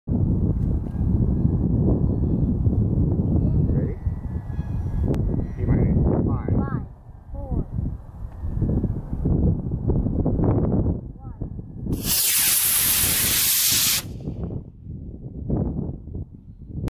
Yellow Star Crayon Maiden Launch
Estes' Yellow Star Crayon rocket with Estes C5 engine.  I didn't see the bent launch lug so the rocket launched successfully but it took the launch rod with it.  Fortunately no one was hurt and rocket was recovered safely.